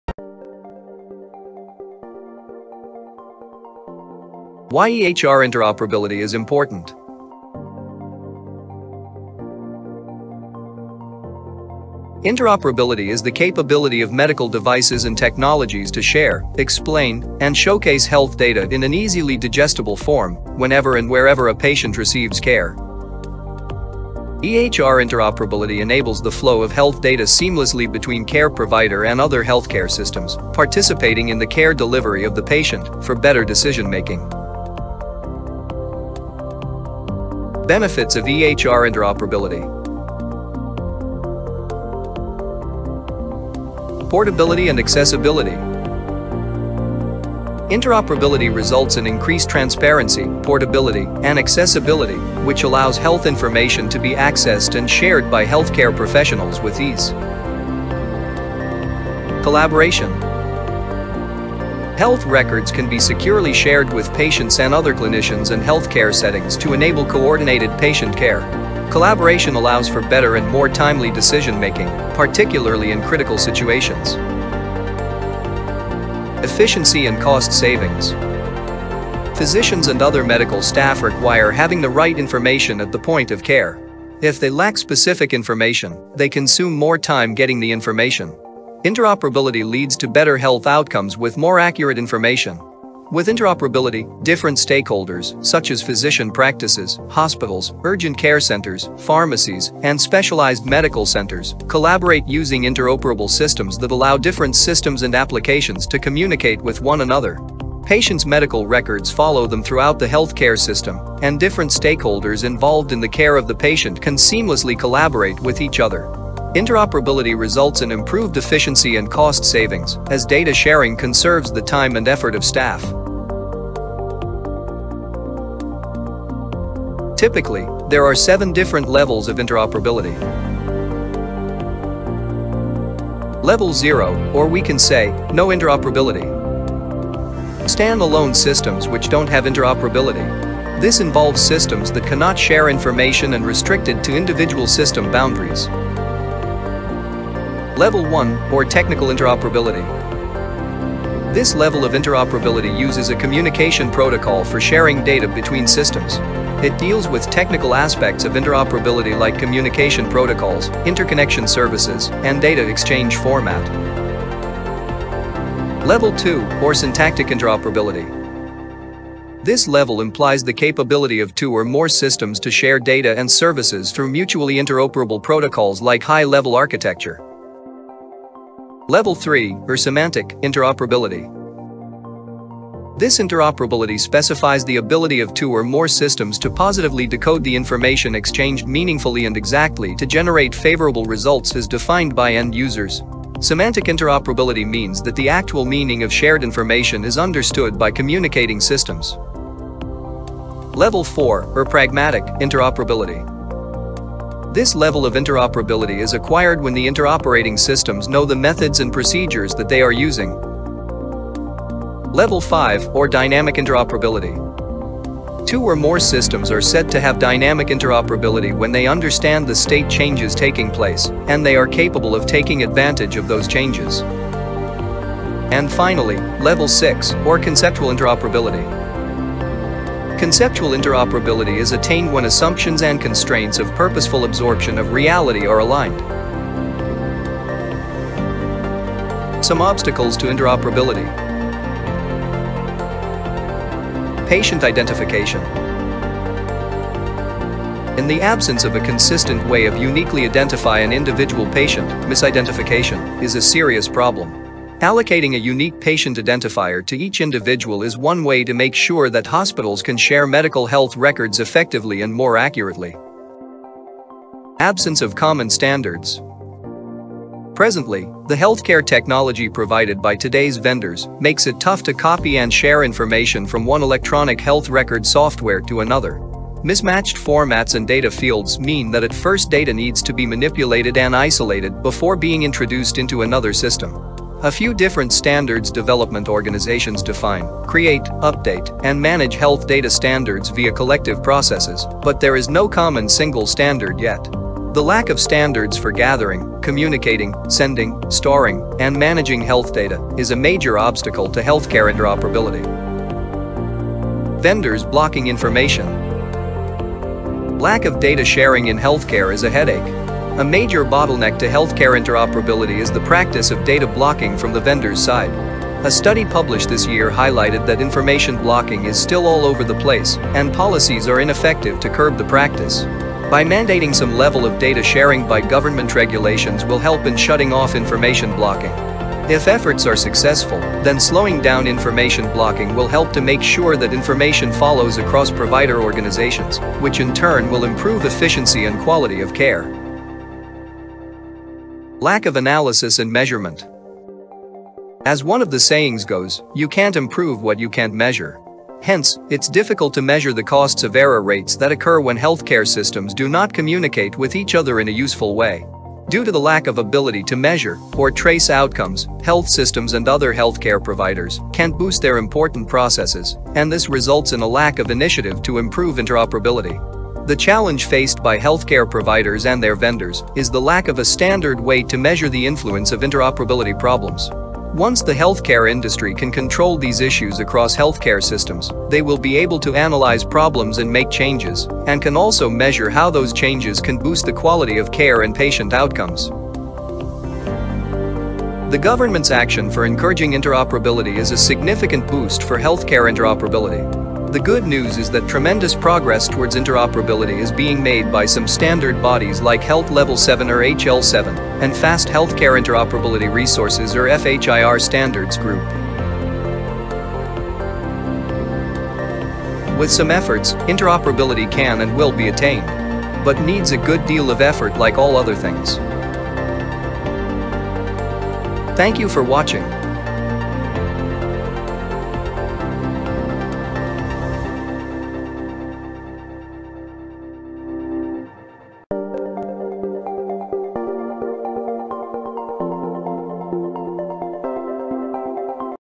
Why is EHR Interoperability Important?
This video describes why EHR Interoperability is important for healthcare.

In healthcare IT, it's really difficult to think of any huge buzzword other than interoperability. Interoperability influences healthcare quality for both - patients and physicians. The major purpose of healthcare interoperability is to enable the flow of health data seamlessly between care-provider and other healthcare systems participating in the care delivery of the patient for better decision making.

Do you know what EMR interoperability implies in healthcare?

EHR or EMR interoperability is the capability of medical devices and technologies to share, explain and showcase health data in an easily digestible form whenever and wherever a patient receives care. Healthcare systems should be able to carefully and actively create and brings meaningful clinical knowledge, decisions, and actions.

Typically, there are seven different levels of interoperability. Keep on reading to know these levels.

- Level 0 or we can say NO Interoperability

    Stand-alone systems which don't have interoperability. This involves systems that cannot share information and restricted to individual system boundaries.

- Level 1 or Technical Interoperability

    This level of interoperability uses a communication protocol for sharing data between systems. It deals with technical aspects of interoperability like communication protocols, interconnection services, and data exchange format.

- Level 2 or Syntactic Interoperability

    This level implies the capability of two or more systems to share data and services through mutually interoperable protocols like High-Level Architecture (HLA).

- Level 3 or Semantic Interoperability

    This interoperability specifies the ability of two or more systems to positively decode the information exchanged meaningfully and exactly to generate favorable results as defined by end-users. Semantic interoperability means that the actual meaning of shared information is understood by communicating systems.

- Level 4 or Pragmatic Interoperability

    This level of interoperability is acquired when the interoperating systems know the methods and procedures that they are using.

- Level 5 or Dynamic Interoperability

    Two or more systems are said to have dynamic interoperability when they understand the state changes taking place and they are capable of taking advantage of those changes.

- Level 6 or Conceptual Interoperability

    Conceptual interoperability is attained when assumptions and constraints of purposeful absorption of reality are aligned. However, interoperability in healthcare has different meaning such as -
        - Interoperability of massages (data) to be shared among healthcare applications.
        - Interoperability of electronic health record (EHR).
        - Interoperability of patients' health records.
        - Interoperability of other business processes.

Webmedy
Wakening Wholesome Wellness